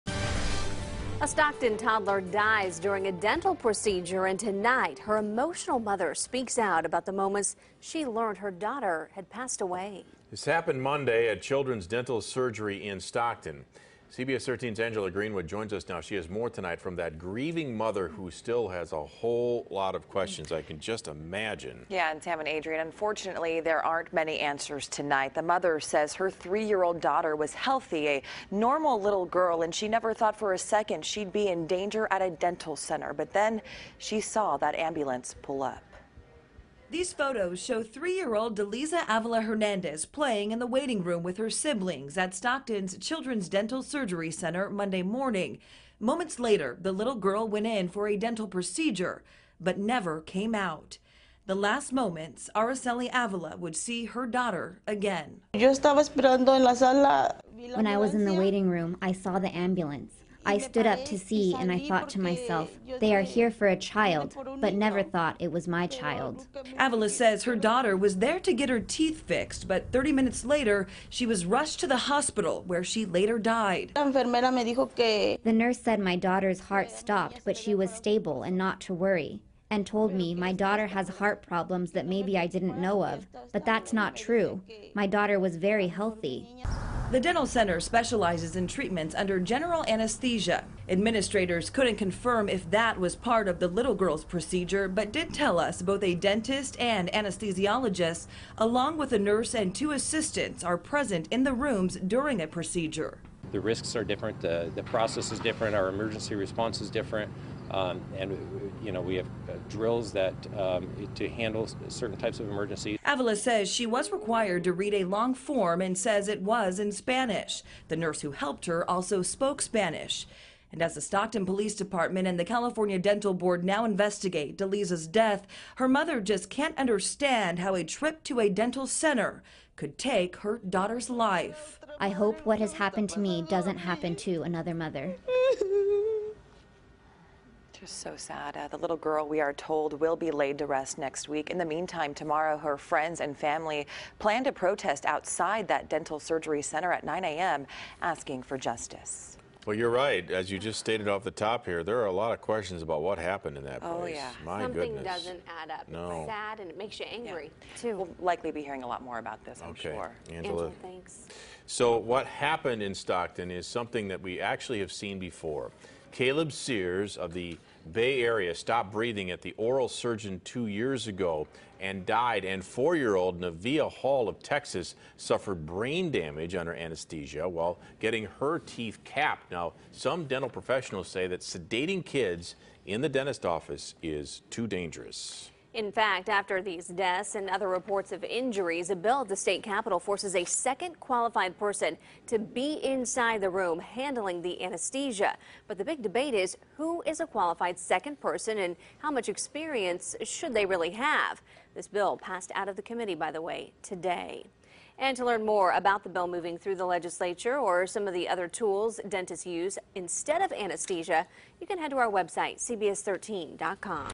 Stockton Toddler Dies During Dental Procedure
Photos taken by her mother, show three-year-old Daleyza Avila-Hernandez playing in the waiting room with her siblings at the dental center.